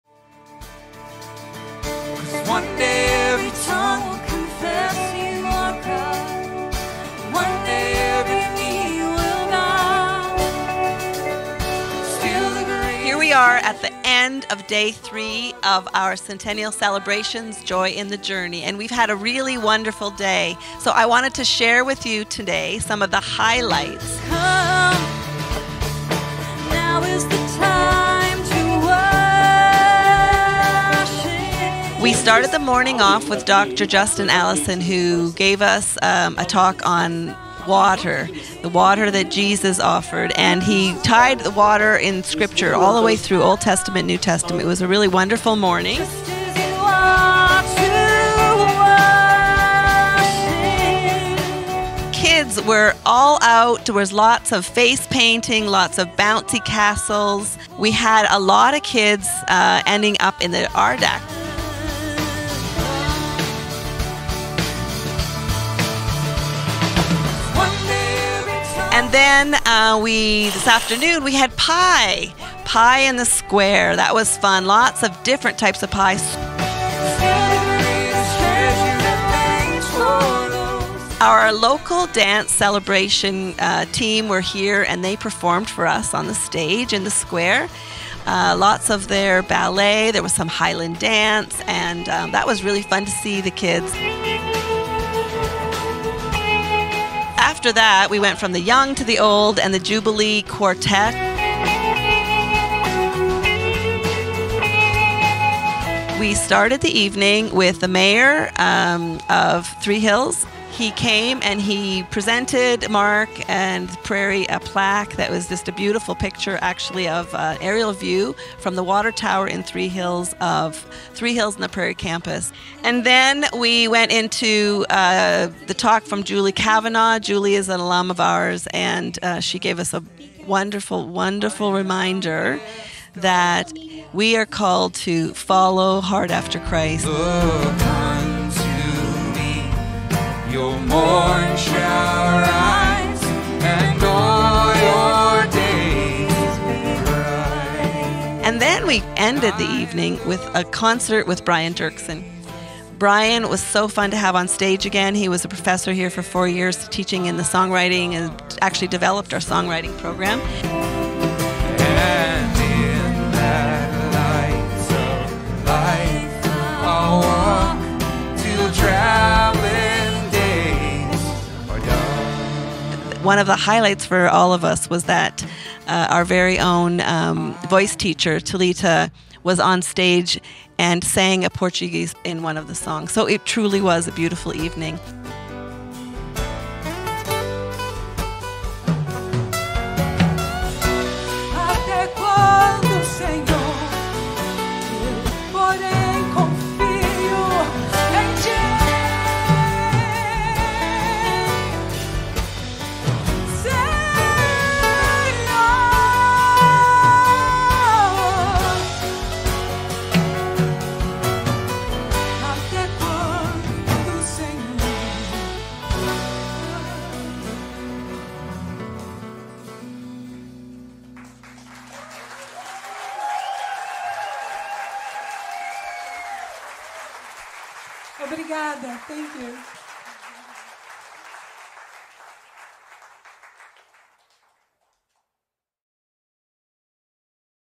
Summary of Day 3
The beautiful weather just added to the warmth of class reunions, pie in the square, and our encouraging sessions in the big white tent.    The final concert was Brian Doerksen, who founded the songwriting program at Prairie.    A great day - the midpoint of our celebration together!